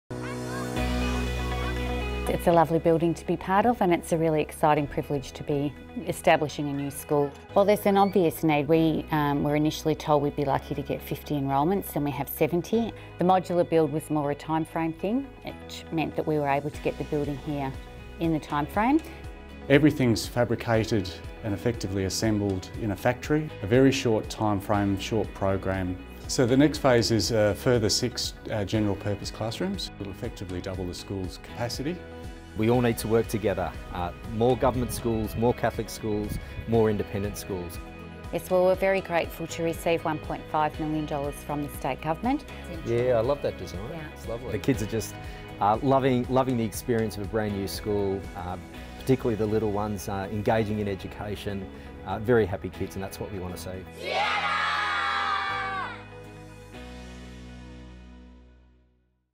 Building Siena Catholic Primary School
A new Catholic primary school is being built in Lucas and the construction progress is right on plan.

In the next stage we'll see a second building with six classrooms, a dedicated art area and toilet facilities.